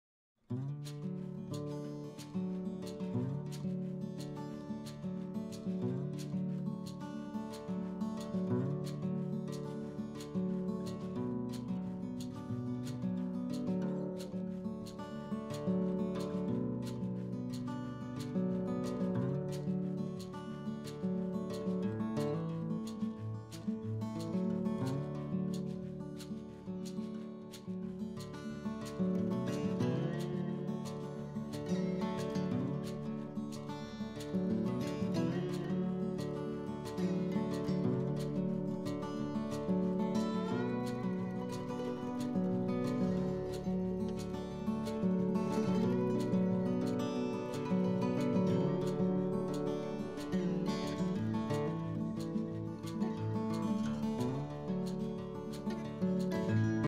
Landscape Watercolor - Wildflower (Arches rough) NAMIL ART
풍경 수채화 - 들꽃 (마스킹 작업, 습식기법, 아르쉬 황목) 남일수채화 NAMIL ART